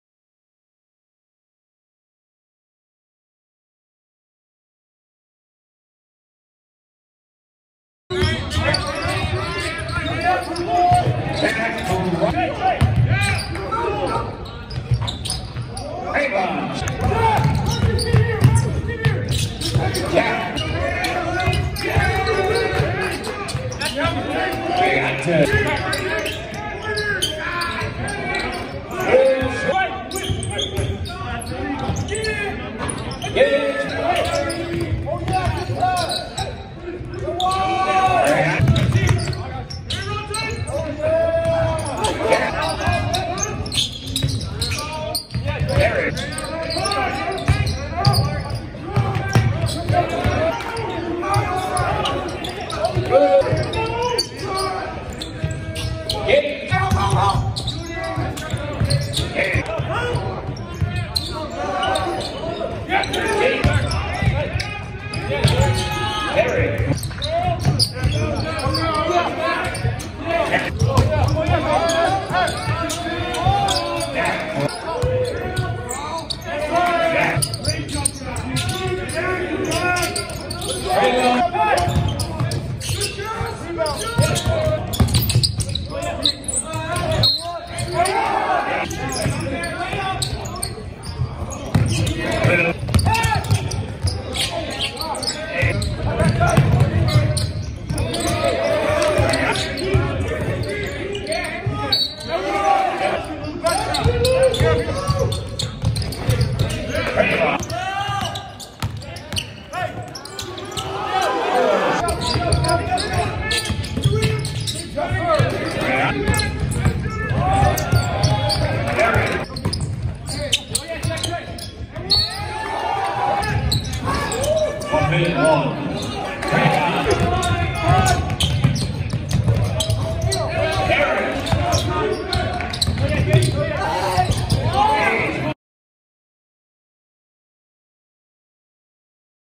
UW-Whitewater Men's Basketball - February 24, 2021
Five players scored in double figures for the University of Wisconsin-Whitewater men's basketball team, but the Warhawks fell just short in a 93-90 overtime defeat against UW-Stout in a Wisconsin Intercollegiate Athletic Conference contest on February 24, 2021, at Kachel Gymnasium. Thumbnail image by Dane Sheehan.